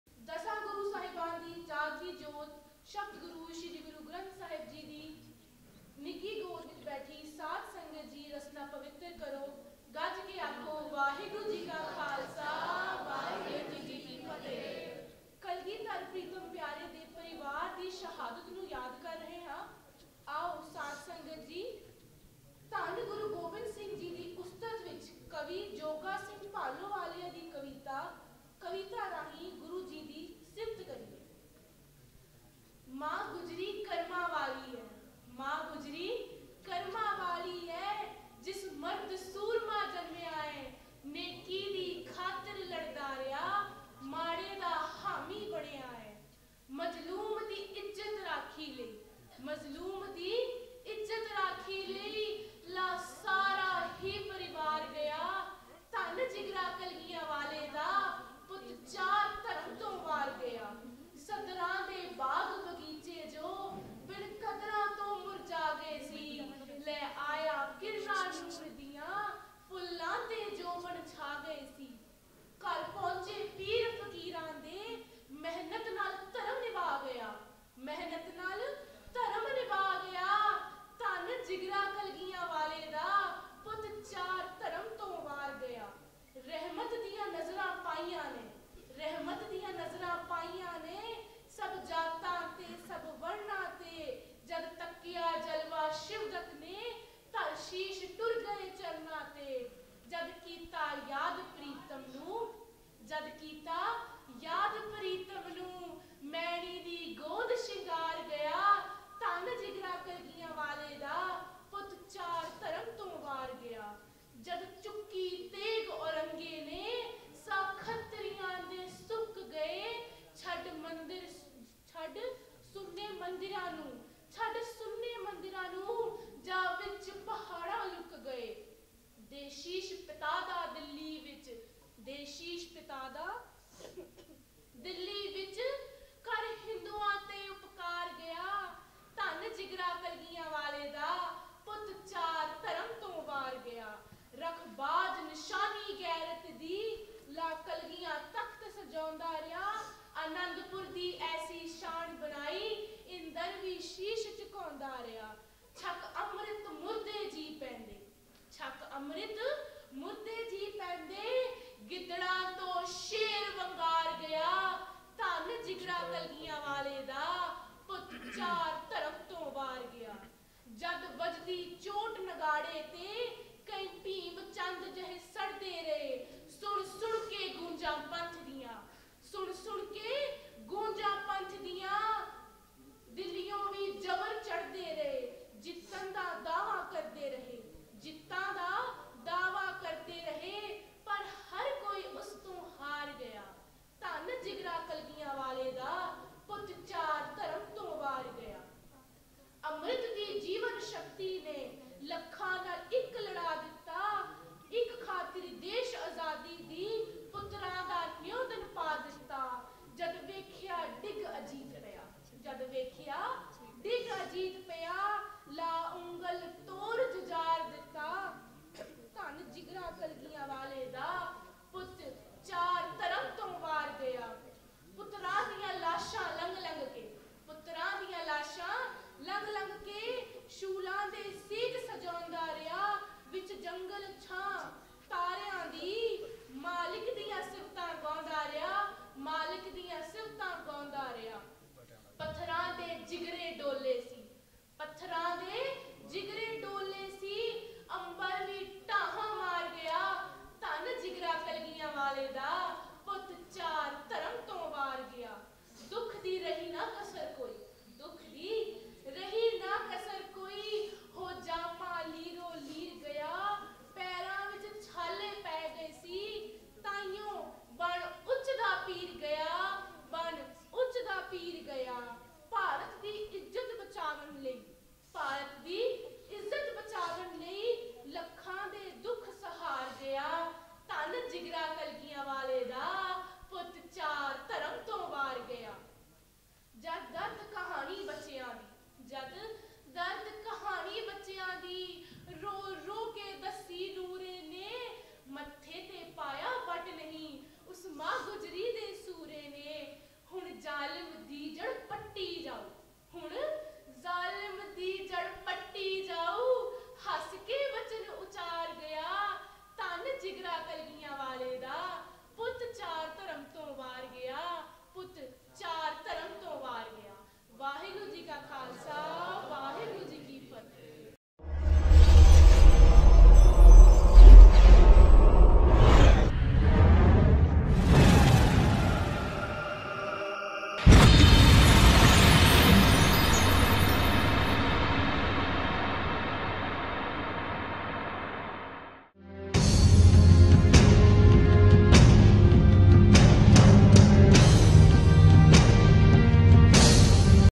ਸਫ਼ਰ-ਏ-ਸ਼ਹਾਦਤ I 25-12-2023 I ਕਵਿਤਾ I ਗੁਰਦੁਆਰਾ ਸ੍ਰੀ ਗੁਰੂ ਸਿੰਘ ਸਭਾ ਅਕਾਲੀ ਕੌਰ ਸਿੰਘ ਨਗਰ ਡਿਗਿਆਣਾ ਜੰਮੂ I
ਸਫ਼ਰ-ਏ-ਸ਼ਹਾਦਤ I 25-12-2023 I ਕਵਿਤਾ I ਗੁਰਦੁਆਰਾ ਸ੍ਰੀ ਗੁਰੂ ਸਿੰਘ ਸਭਾ ਅਕਾਲੀ ਕੌਰ ਸਿੰਘ ਨਗਰ ਡਿਗਿਆਣਾ ਜੰਮੂ  I Gurdwara Sri Guru Singh Sabha Akali Kour Singh Nagar Digiana Jammu I Kavita I VOSP TV I Voice of Sikh Panth TV I